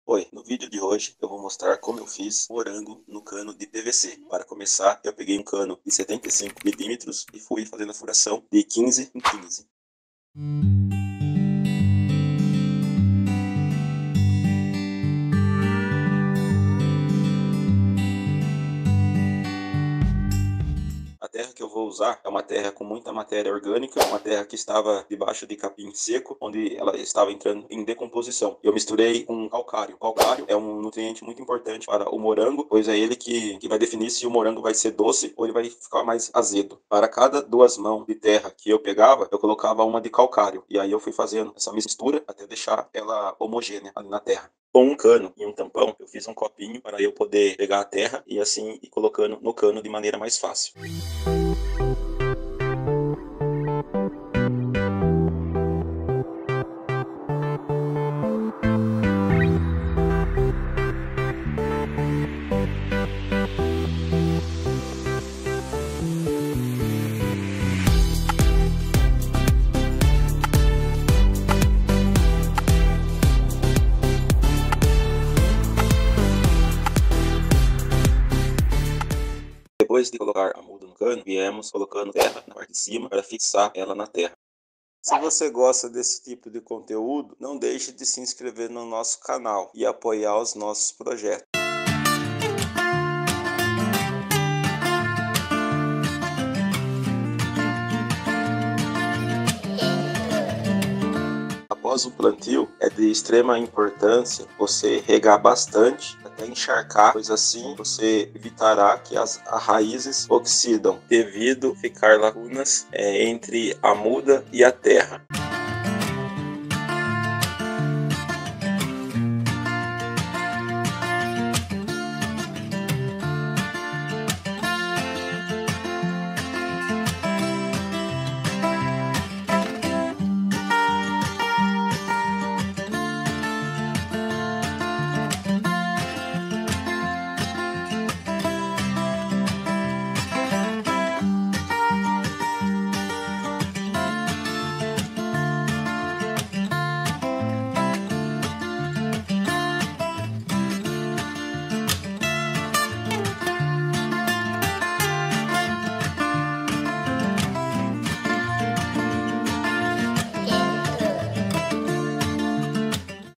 Como plantar morango em cano de pvc, rápido e fácil!
🍓🌱 Cultivar morangos em canos de PVC é uma maneira inovadora e prática de desfrutar dessas deliciosas frutas em seu próprio espaço. Com essa técnica, você pode aproveitar ao máximo seu espaço vertical, cultivando morangos em uma estrutura compacta e eficiente. 🌿🌼 O cultivo em canos de PVC oferece diversos benefícios, como economia de espaço, controle adequado do solo e fácil manutenção. Além disso, permite um melhor acesso aos morangos, evitando que eles fiquem em contato direto com o solo e reduzindo o risco de pragas e doenças. 🌱💦 Com cuidados adequados, como rega regular, adubação e controle de pragas, você pode colher morangos frescos, suculentos e saborosos em sua própria casa ou jardim. Não há nada mais satisfatório do que saborear morangos cultivados com carinho e colhidos diretamente da planta. 🍓🌿 Então, por que não experimentar o cultivo de morangos em canos de PVC e desfrutar de uma colheita abundante e deliciosa? Aproveite essa técnica criativa e transforme seu espaço em um verdadeiro oásis de morangos frescos! 🌱🍓✨